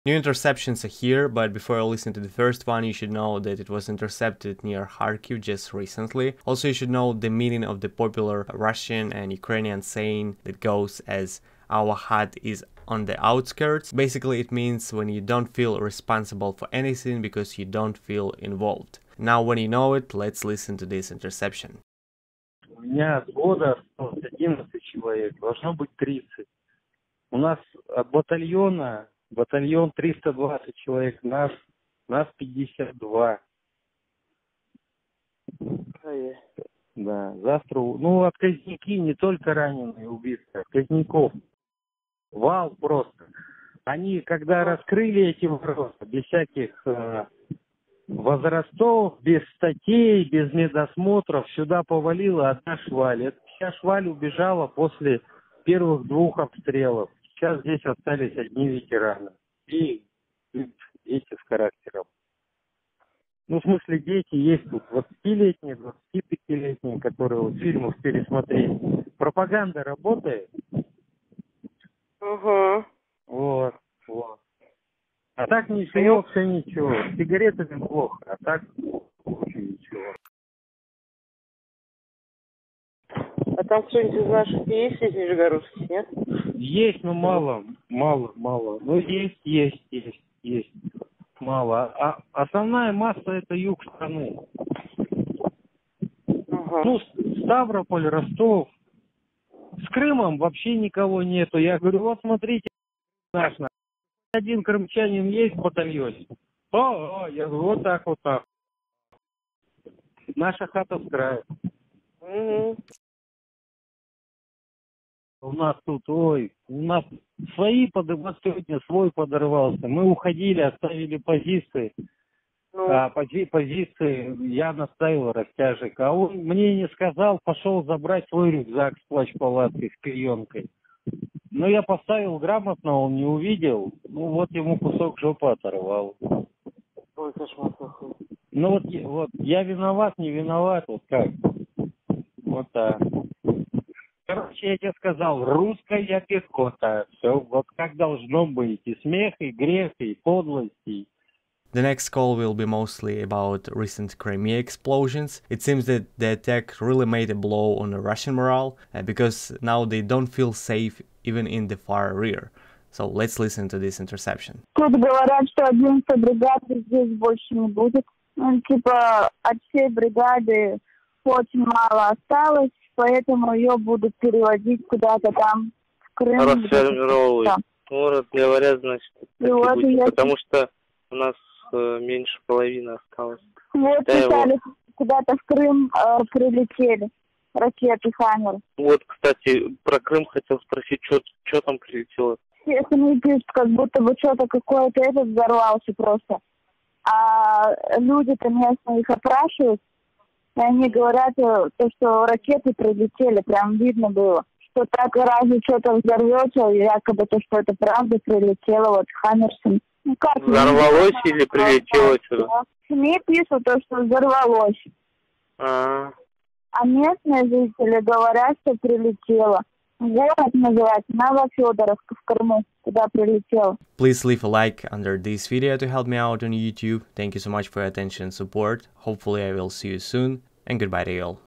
“Stupid Russian Patriotic Movies Make Them Go To War” - Russian Veteran In Ukraine About Youngsters
New insights from the frontline are waiting for you! You’ll hear 2 freshly intercepted phone calls translated by me to English. Also thanks to all of you who became a member of my channel!